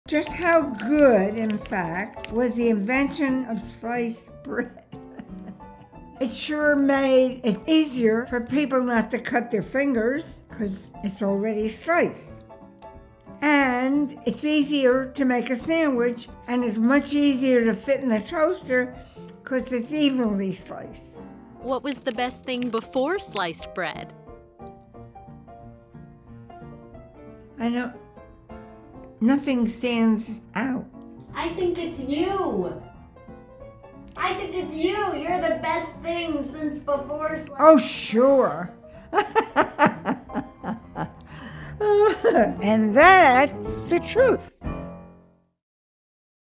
100-Year-Old Recalls When Sliced Bread Was Invented
Great-Grandma Ruth remembers a time when sliced bread was new, better than I recall what I had for dinner last night!

Episodes by  @GoodPorpoise   Productions (Elisabeth Ness & Ed Nescot) and Brianne Neira.